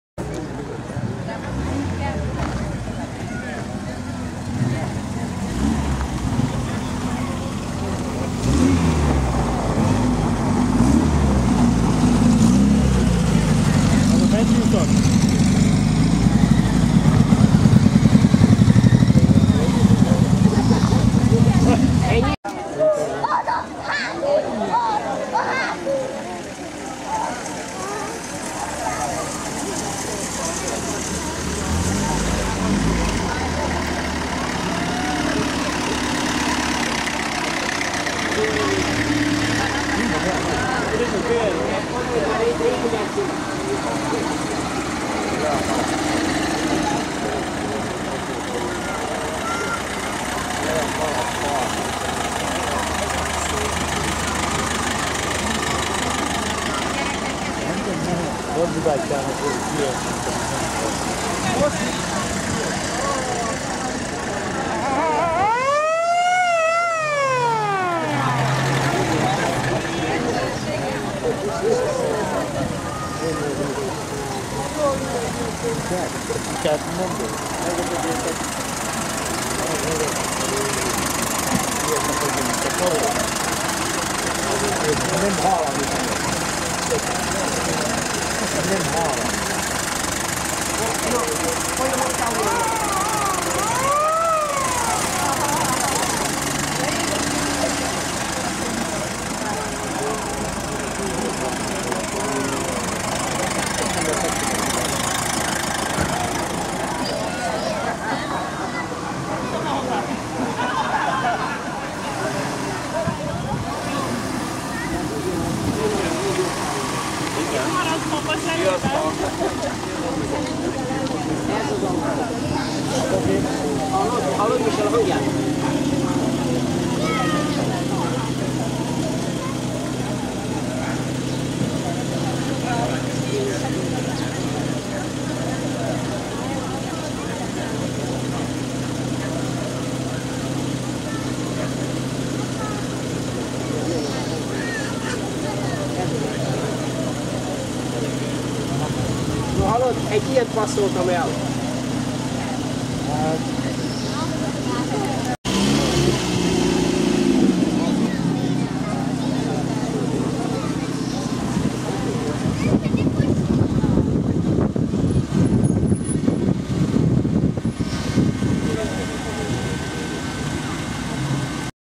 Veterán autók és motorok XIII. találkozója 20.5.2018 Borsi - Stretnutie historických áut Borša
Veterán autók és motorok XIII. találkozója- Lévai György Emléktúra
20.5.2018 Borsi
Stretnutie historických áut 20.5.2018 Borša